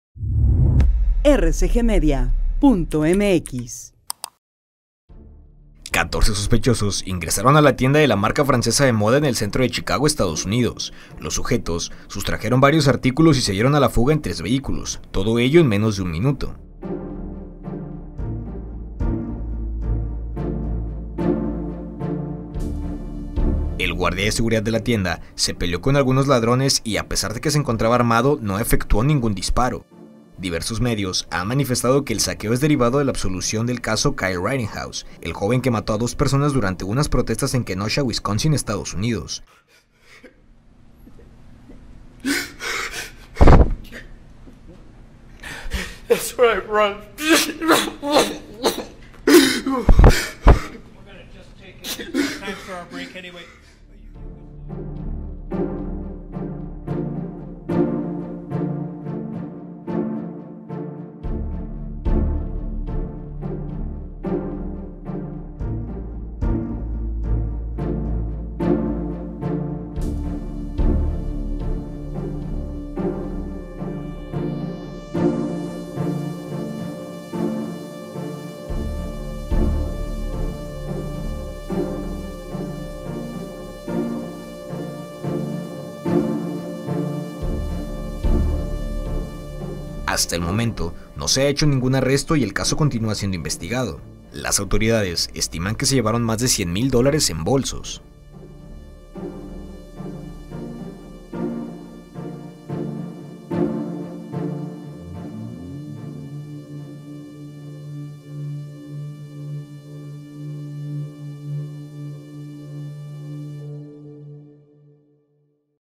¡ROBO MASIVO en tienda LOUIS VUITTON en menos de un minuto! | Chicago, EUA
El Departamento de Policía de Oak Brook determinó que 14 sospechosos ingresaron a la tienda de la marca francesa de moda, substrajeron varios artículos y se dieron a la fuga en tres vehículos, todo ello en menos de un minuto en Chicago, EUA.